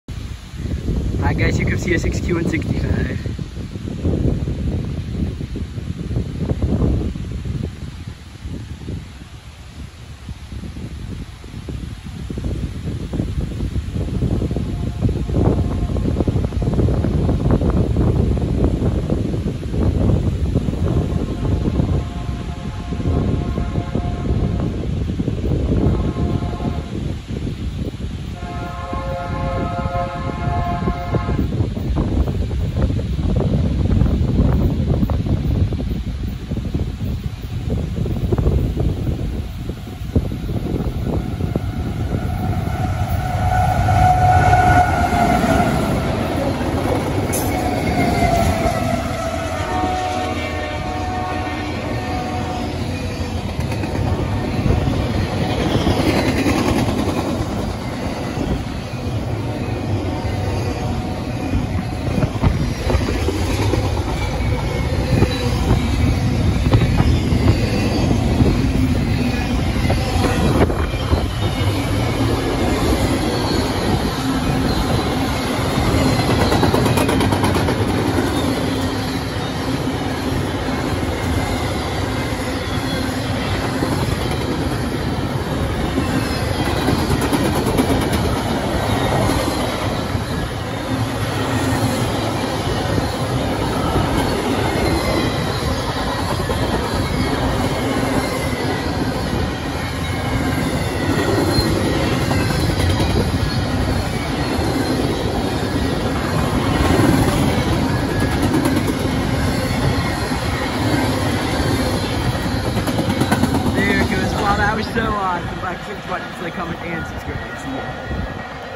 Leasers Galore! CEFX GP38-3s & CITX SD40-2 Trail between two CP Locomotives on CSX Q165!
This was such an awesome catch! Huge thanks to the many people who helped us track this thing down!